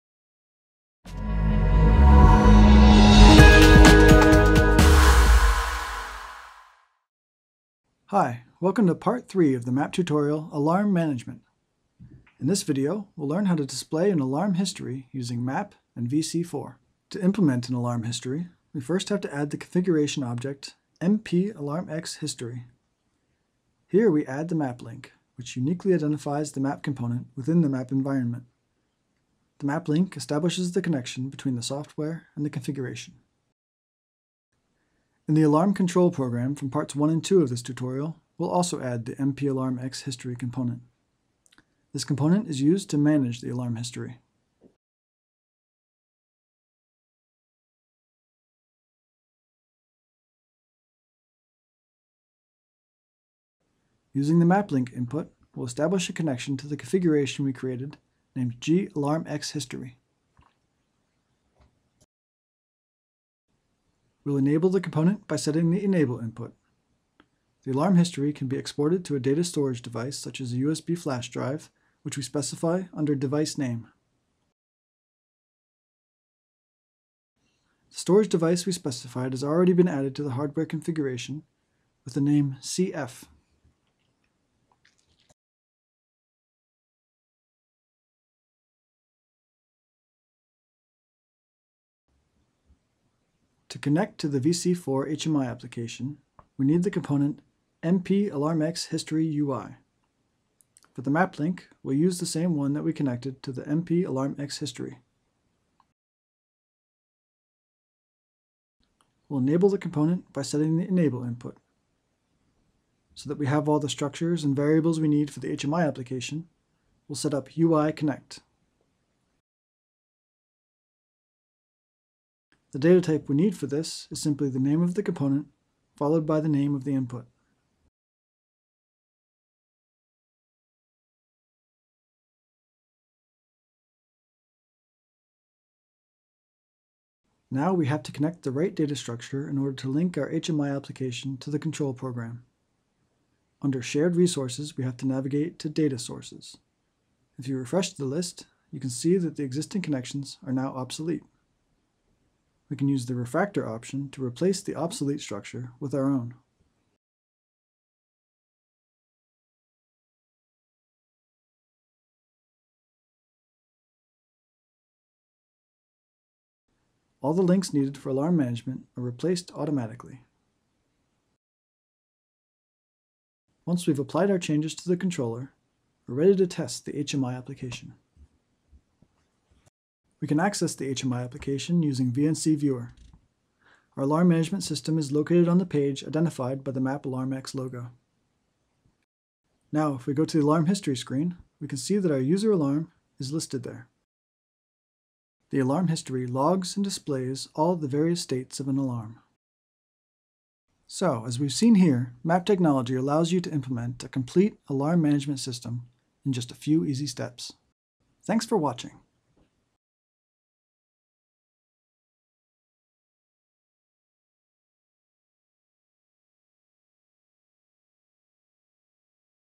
mapp AlarmX 3 – How to get the alarm history [sub: EN, DE, PL, PT-BR, RU]
MpAlarmX collects and manages both mapp alarms and user alarms. To implement an alarm history, we first have to add the configuration object "MpAlarmXHistory". This function block can be used to export the alarm history to a specified data storage device. Here, we add the mapp Link, which uniquely identifies the mapp component within the mapp environment. The mapp Link establishes the connection between the software and the configuration.

This is part 3 of the mapp tutorial "Alarm management". In this video we'll learn how to display an alarm history using mapp and VC4.

A structured documentation is available in our Automation Studio help center under “MpAlarmX – Complete alarm management”.